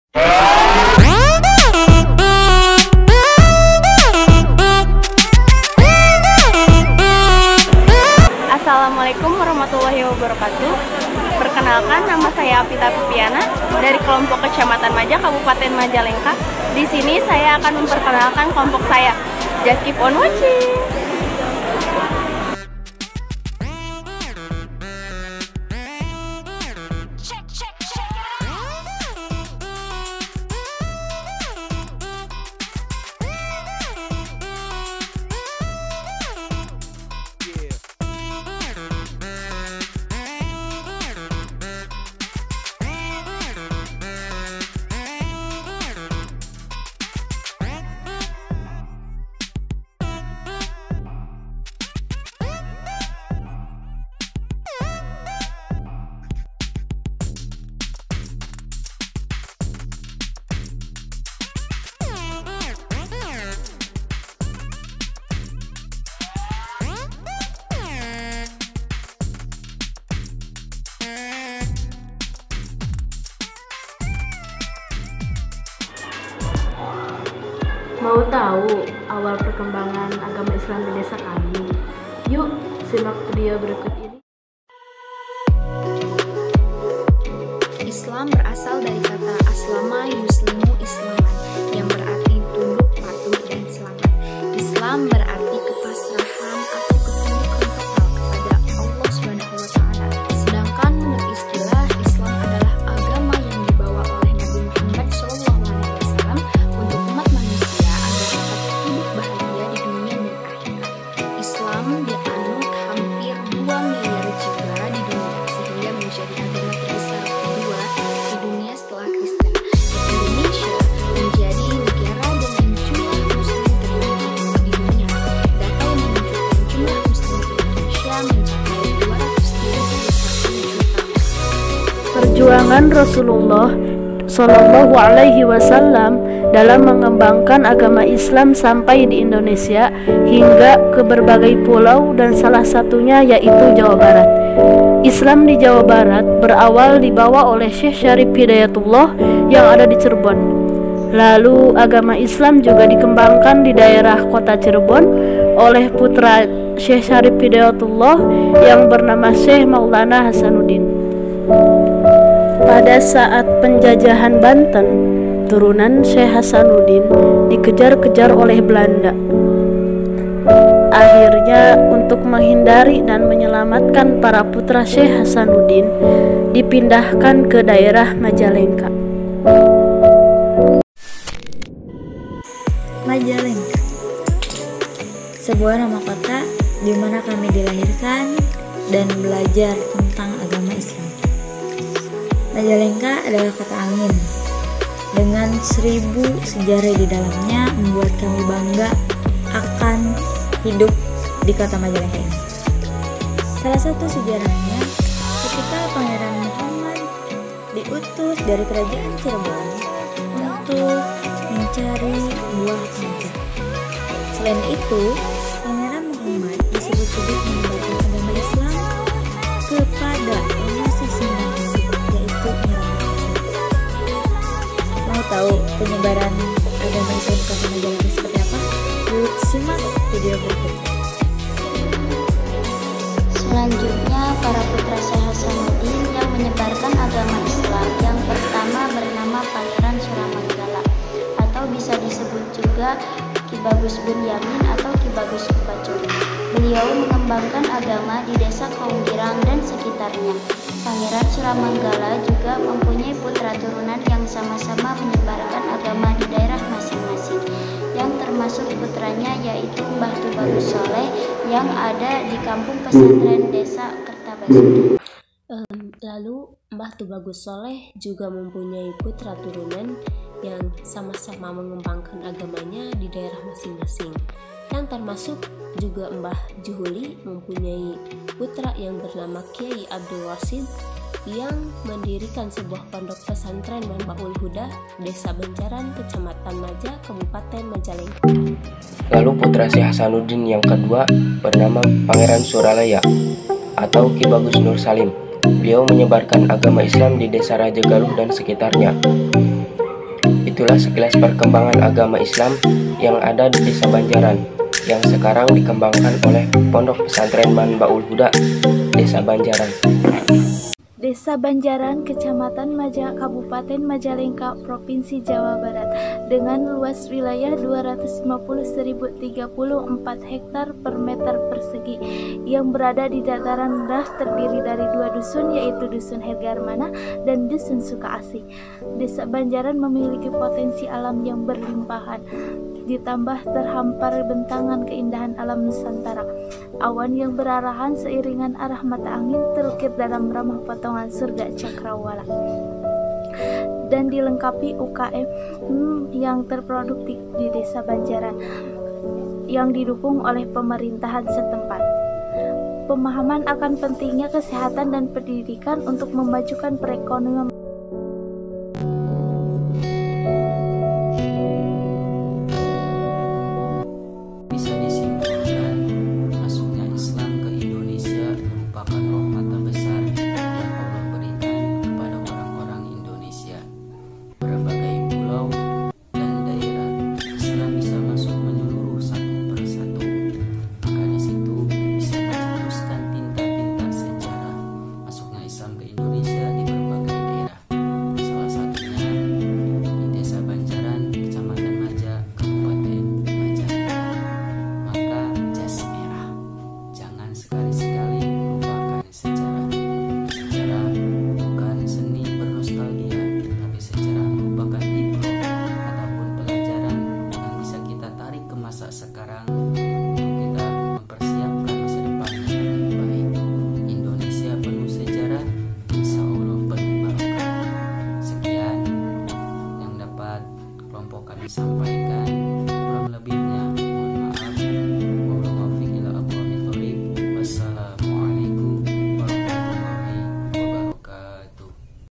Sejarah keislaman| perekonomian| Di Desa Banjaran Kec. Maja Majalengka pendidikan ekonomi#semester1
assalamualaikum sebagai memenuhi tugas Matkul PAI
dosen pengampu : Salman Alfarisi,Mpd.I

kelompok kabupaten Majalengka
1. Avita Viviana
2. Eem Nurhalimah
3. Efih Ulfa Fauziyyah
4. Dede Nurul Ikhsan
5. Rini Nur'aeni
6. Rida Fauziah
7, Ilyas Irwansyah
8. Cici Ikhlasul Amanah
9. Mala Fitri Tasa
10. Hilman Abdul Halim